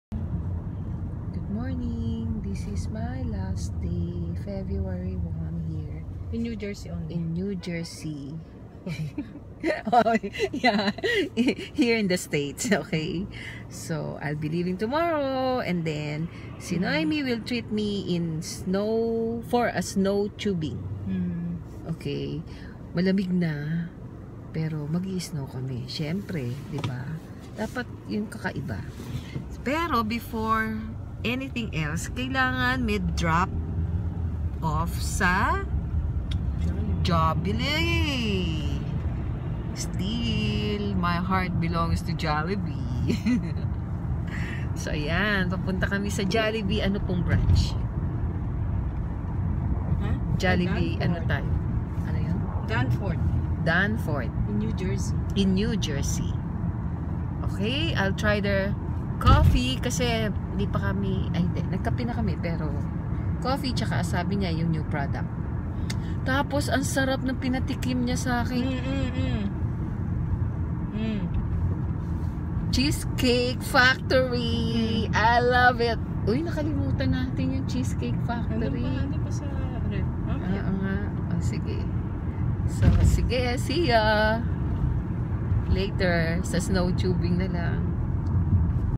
Snowtubing 101 Camelback Resort
Joyful for this privileged last Jan 2018,  God you are awesome! Thanks to the generosity and love of Noeme and Angie